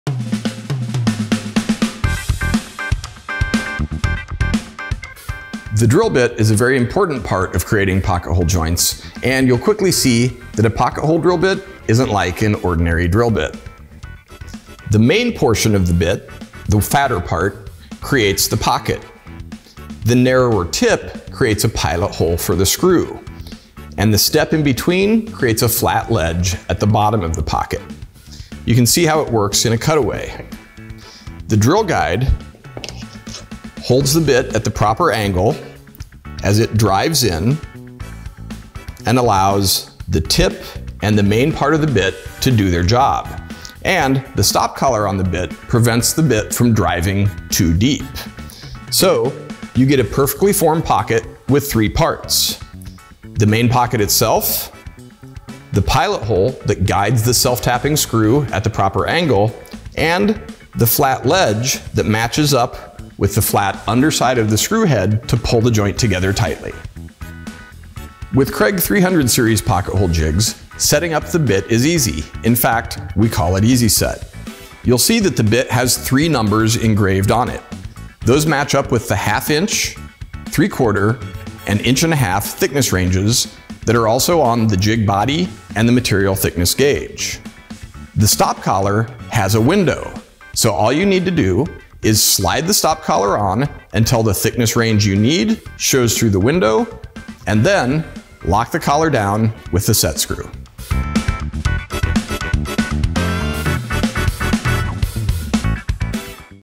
Kreg® 300-Series Pocket-Hole Jigs: Setting the Stop Collar on the Easy-Set Bit
Kreg 300-Series Pocket-Hole Jigs are designed to be easy to use, so you’ll get great project results. Here, we’ll help you learn more about your jig by showing how to set the stop collar on the Easy-Set Pocket-Hole Drill Bit.

KPHA330 Kreg® Easy-Set Stop Collar & Material Gauge/Hex Wrench Kit

KPHA308 Kreg® Easy-Set Drill Bit with Stop Collar & Gauge/Hex Wrench